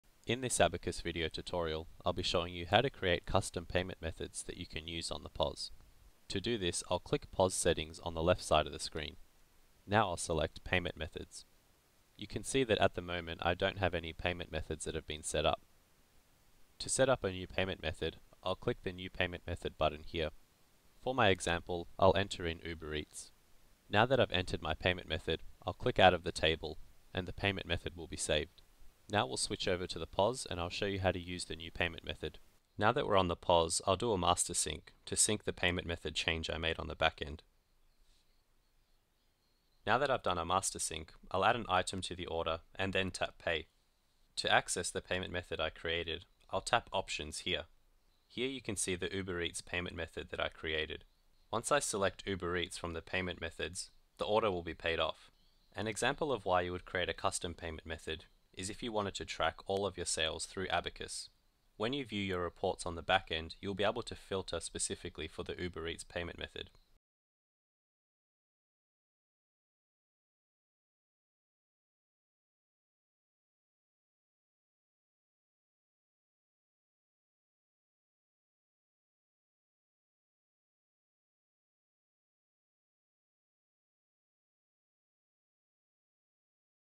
17. Payment methods
In the seventeenth video of our backend training series, we'll cover the following:

- Creating payment methods on the backend
- Using payment methods on the POS